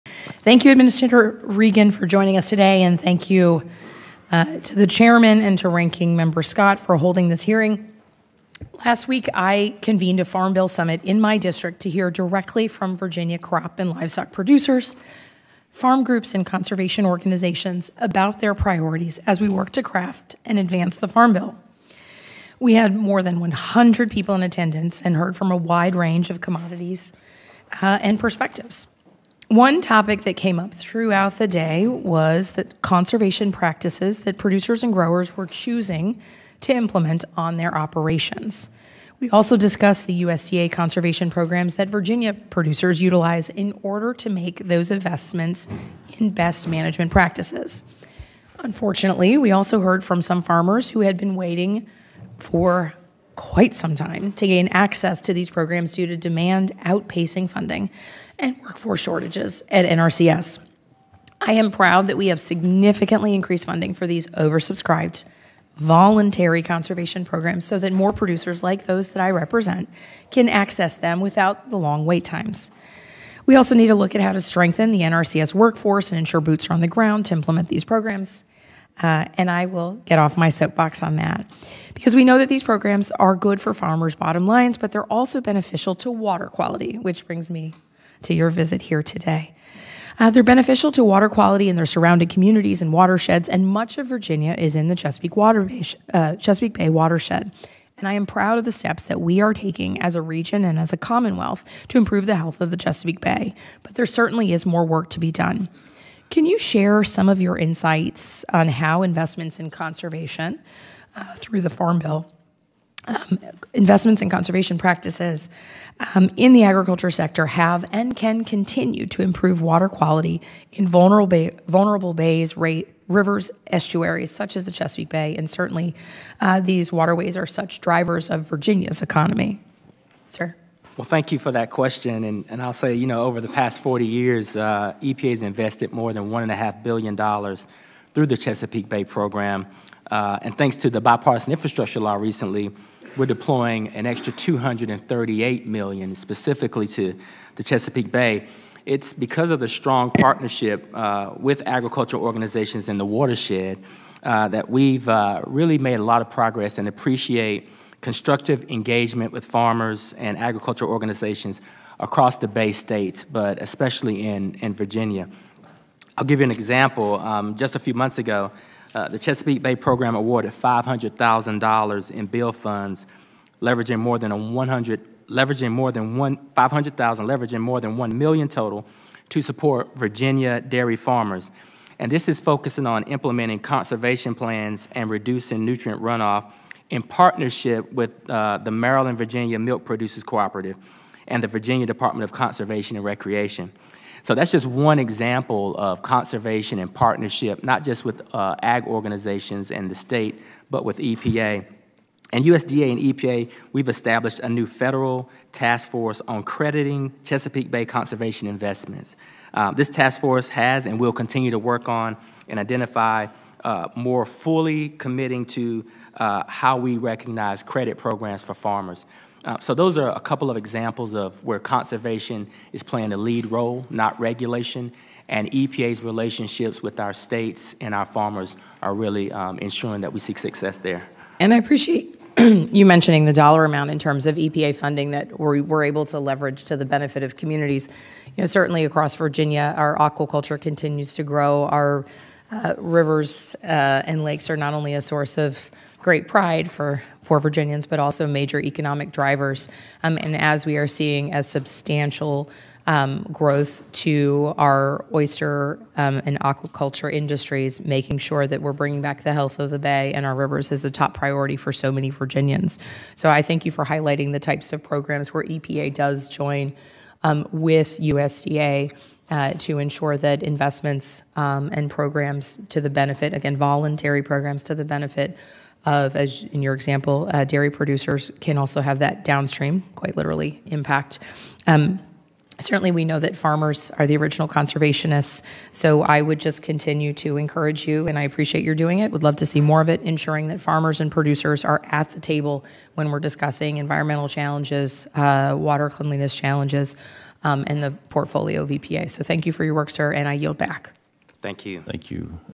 Spanberger Questions U.S. EPA Administrator Regan on Conservation Programs, Chesapeake Bay Watershed
U.S. House Agriculture Committee, For the purpose of receiving testimony from The Honorable Michael Regan, Administrator, U.S. Environmental Protection Agency: 4/19/2023